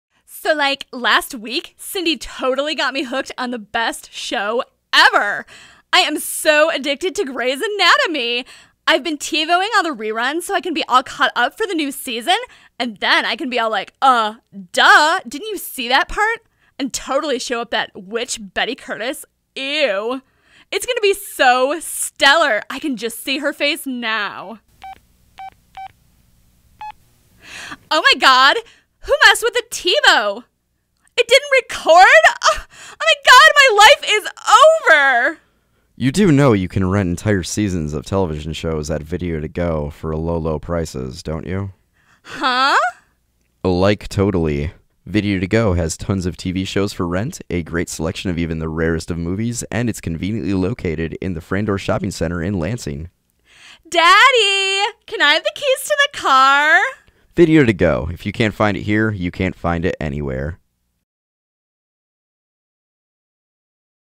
Copywriting/Production Assignment
I am especially fond of this project because I was able to display my production abilities as well as my ability to write copy. I wrote out the script and then recorded my talented LCC Radio co-worker Jennifer doing some great voice acting.